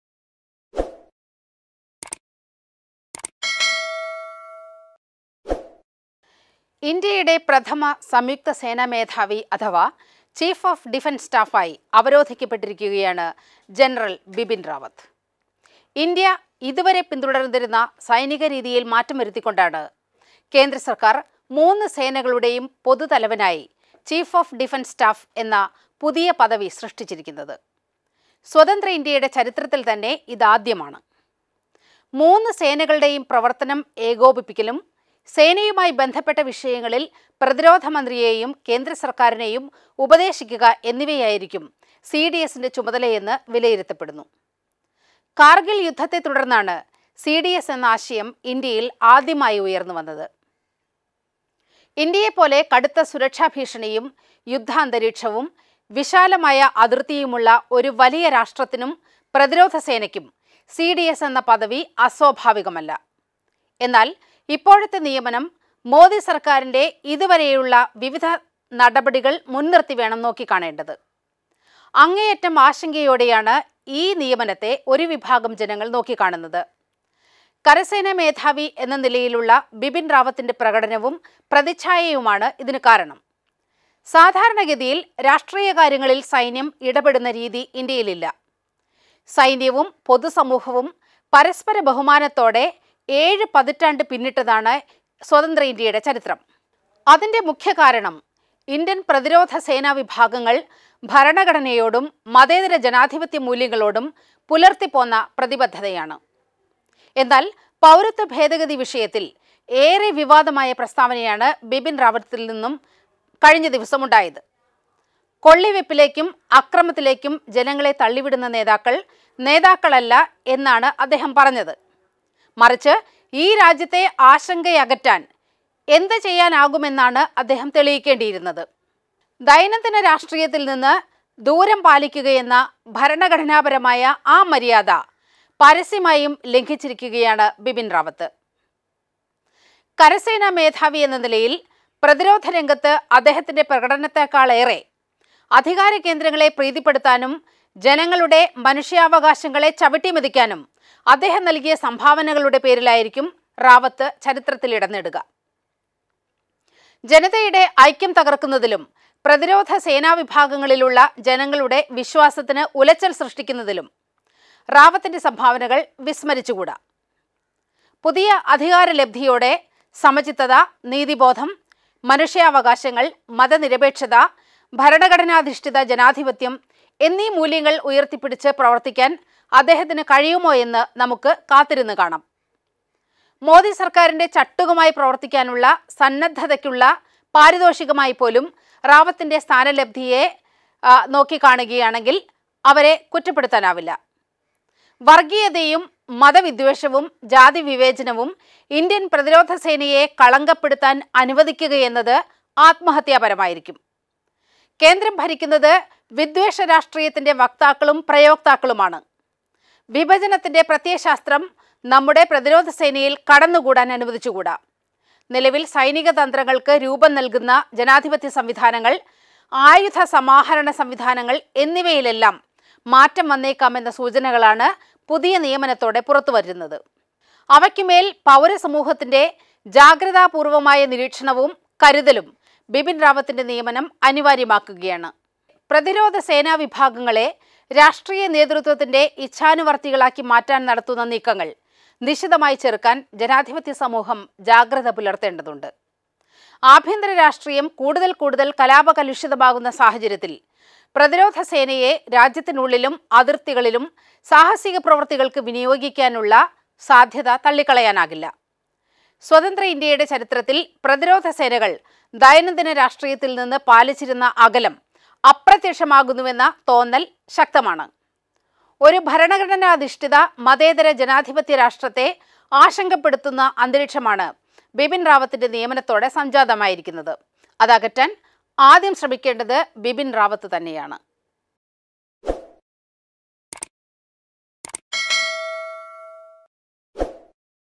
ആശങ്കയോടെയാണ് ഈ നിയമനത്തെ ജനങ്ങള്‍ നോക്കിക്കാണുന്നത്‌ | Bipin Rawat appointment
ഇന്ത്യയുടെ പ്രഥമ സംയുക്ത സേനാമേധാവി അഥവാ ചീഫ് ഓഫ് ഡിഫന്‍സ് സ്റ്റാഫ്ആയി അവരോധിക്കപ്പെട്ടിരിക്കയാണ്  ജനറല്‍ ബിപിന്‍ റാവത്ത്  . ഇന്ത്യ ഇതുവരെ പിന്തുടര്‍ന്നിരുന്ന സൈനിക രീതിയില്‍ മാറ്റം വരുത്തിക്കൊണ്ടാണ് കേന്ദ്രസര്‍ക്കാര്‍ മൂന്ന് സേനകളുടെയും പൊതു തലവനായി ചീഫ് ഓഫ് ഡിഫന്‍സ് സ്റ്റാഫ് എന്ന പദവി സൃഷ്ടിച്ചിരിക്കുന്നത്. സ്വതന്ത്ര ഇന്ത്യയുടെ ചരിത്രത്തില്‍ത്തന്നെ ഇതാദ്യം